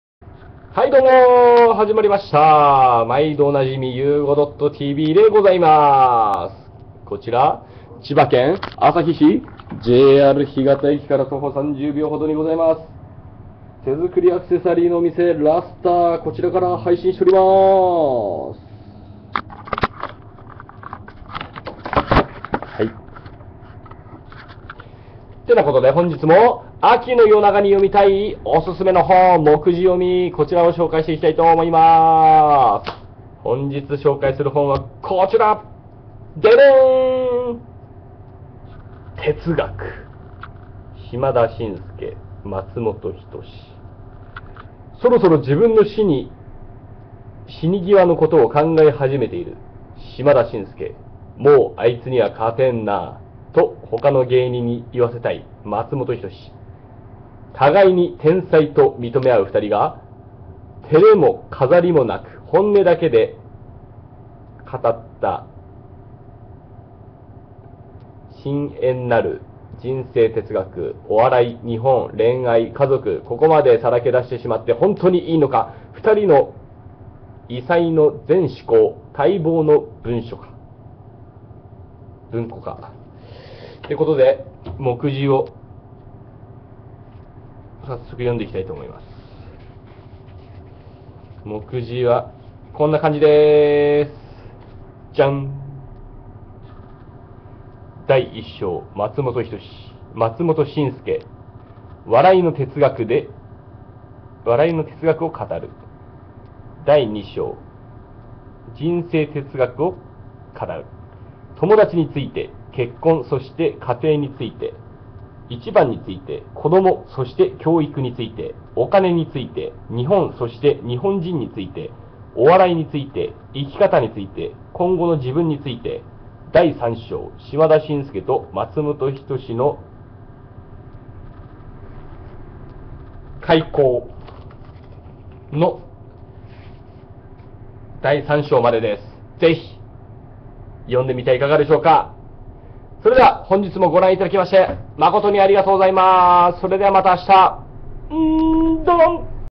第7冊 オススメ本（もくじ読み） 島田紳助 松本人志 哲学
「そろそろ自分の死に際のことを考え始めている」島田紳助。「『もうあいつには勝てんな』と他の芸人にいわせたい」松本人志。互いに“天才”と認め合う二人が、照れも飾りもなく本音だけで綴った深遠なる「人生哲学」。お笑い、日本、恋愛、家族…ここまでさらけ出してしまって、本当にいいのか?二人の異才の全思考、待望の文庫化。